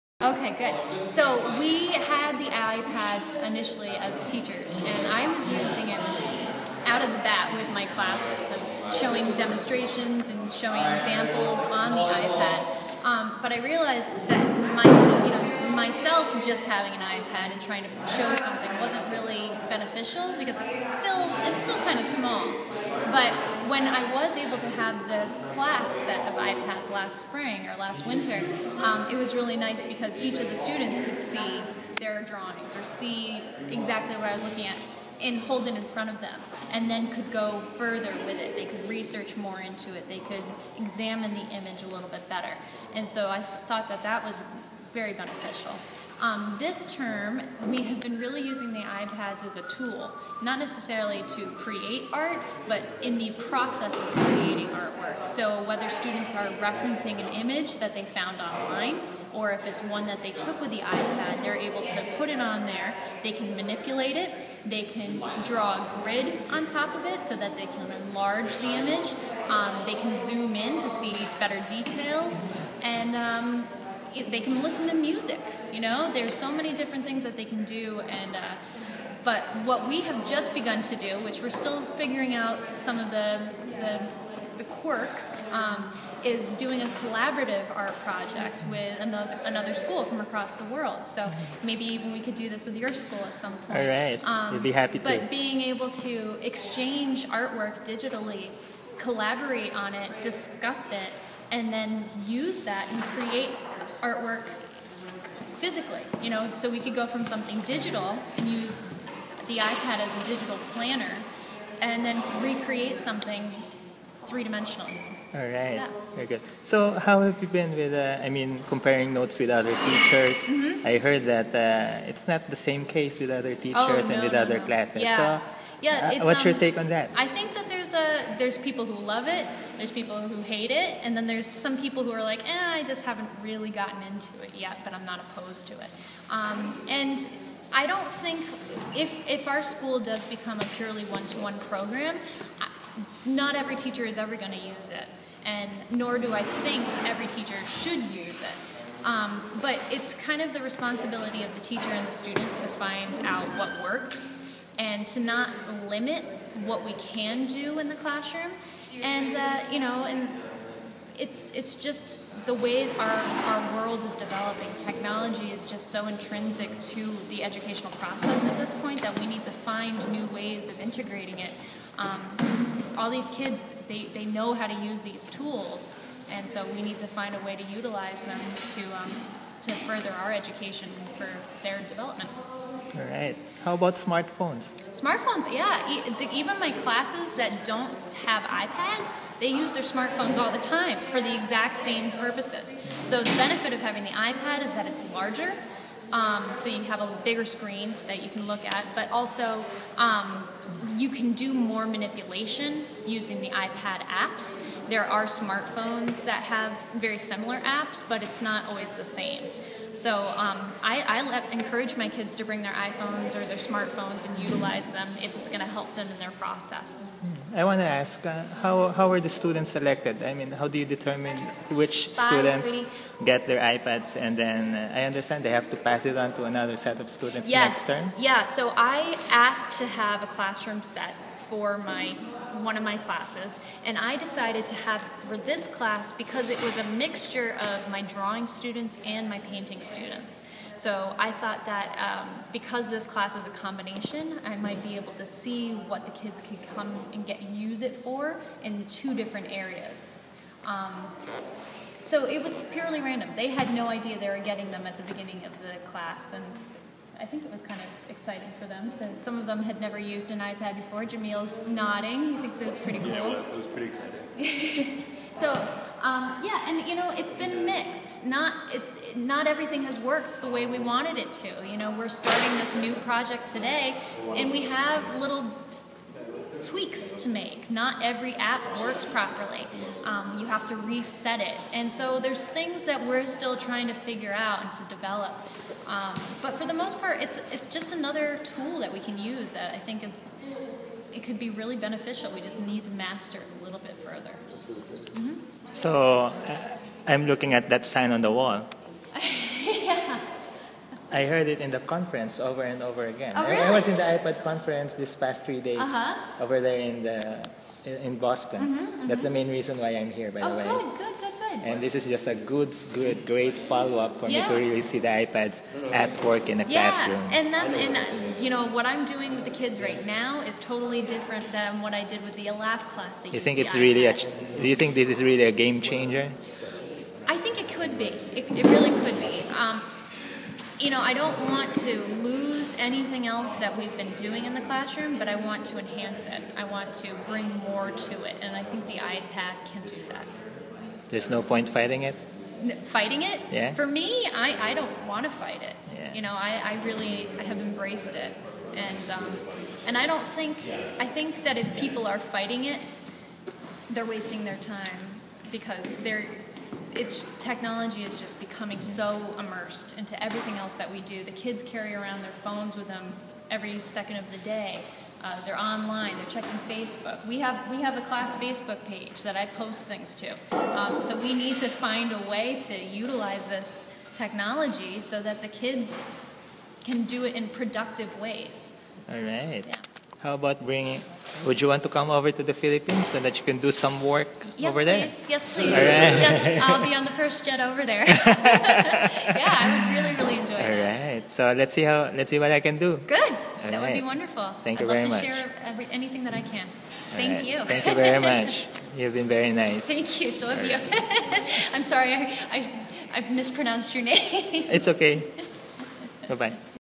Rebecca Barsi interview
Rebecca Barsi, Art Instructor at the Phillips Exeter Academy discusses about how they have been using iPads in classrooms.  Interviewed by JV Salayo of Miriam College last November 9, 2012.